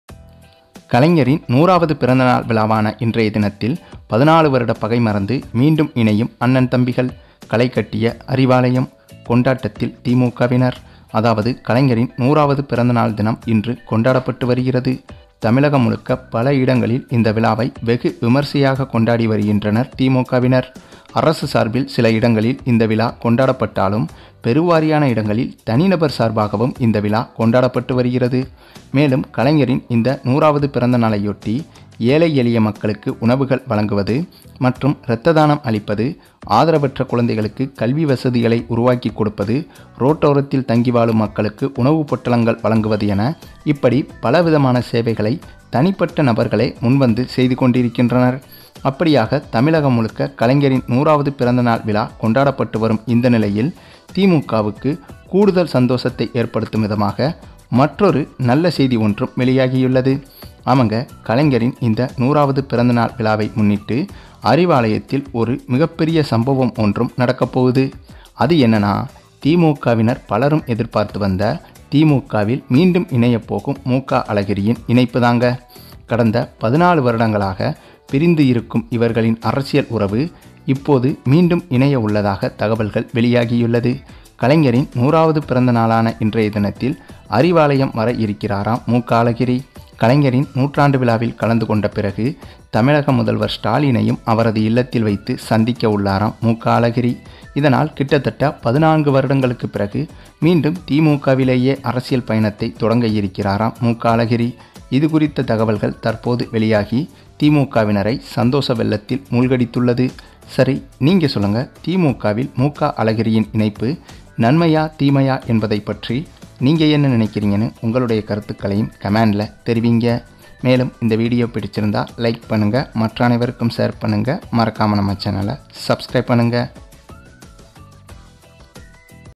கலைஞர் பிறந்த நாள் ! ஸ்டாலின் - மு.க.அழகிரி திடீர் சந்திப்பு : Kalaingar 100
kalaingar 100th celebration videos news